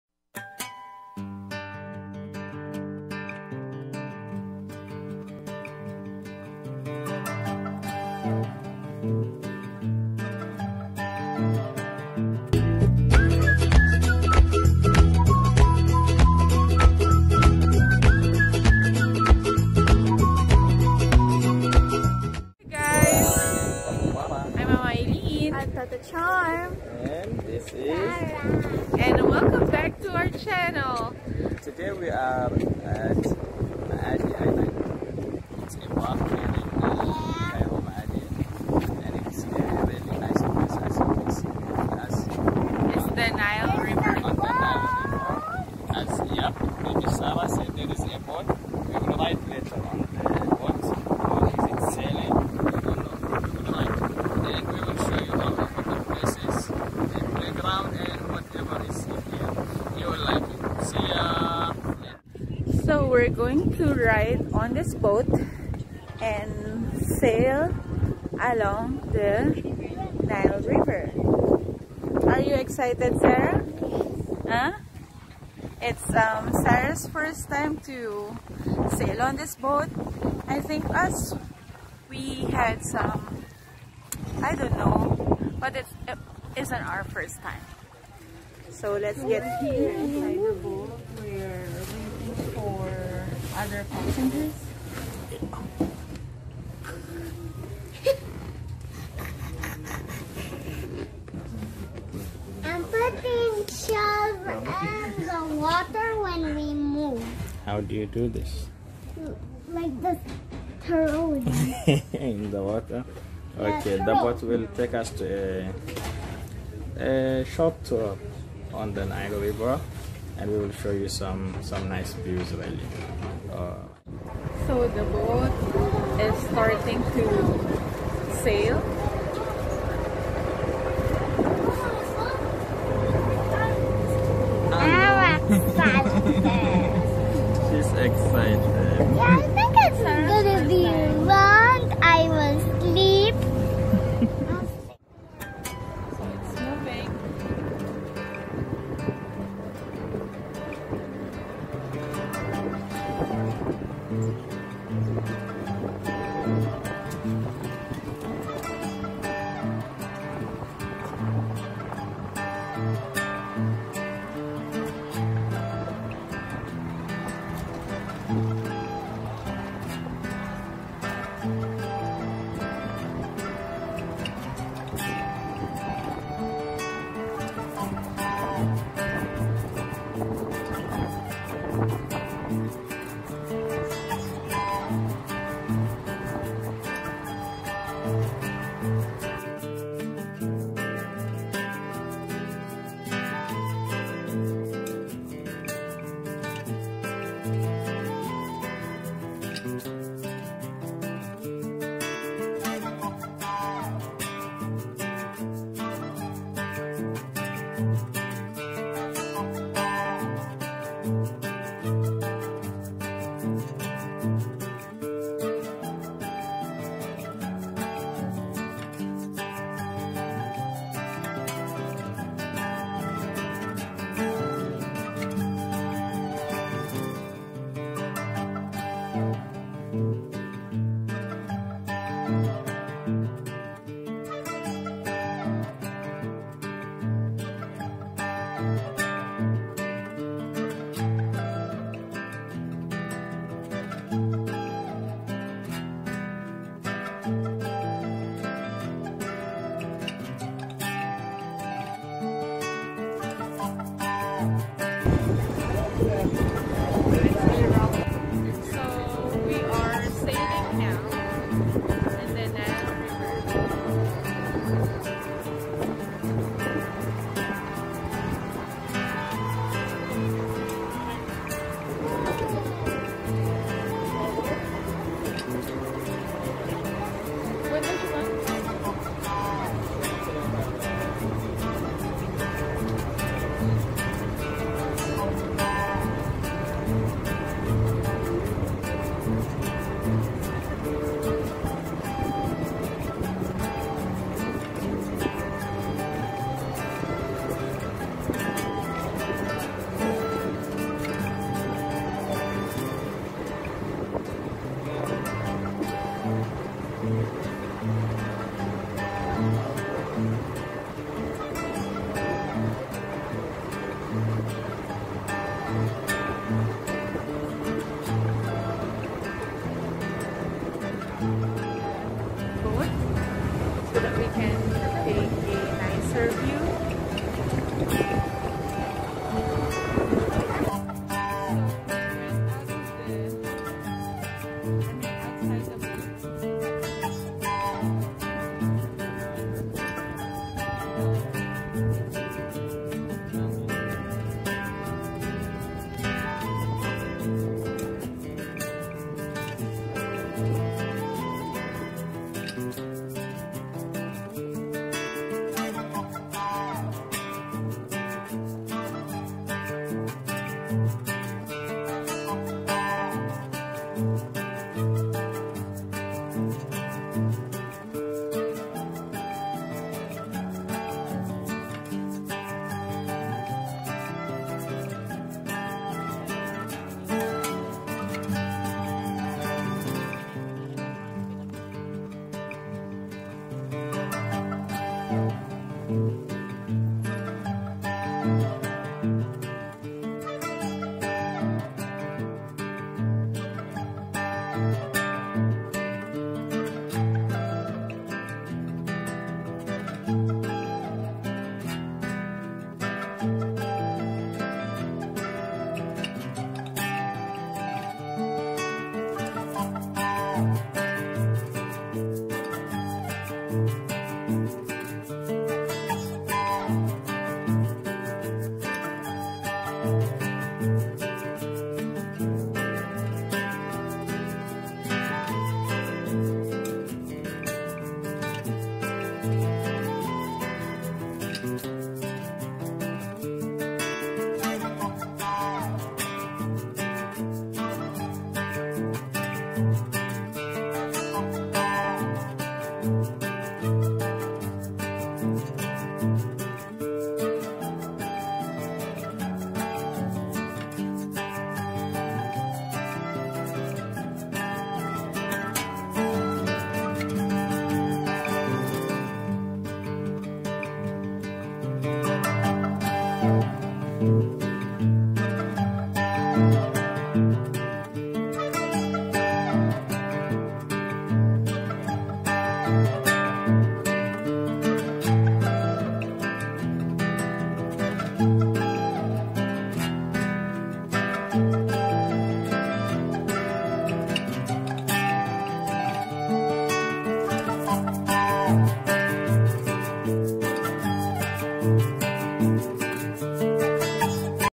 Sailing along the NILE RIVER
Hello everyone! It's such a sunny day so let's go and sail along the Nile River!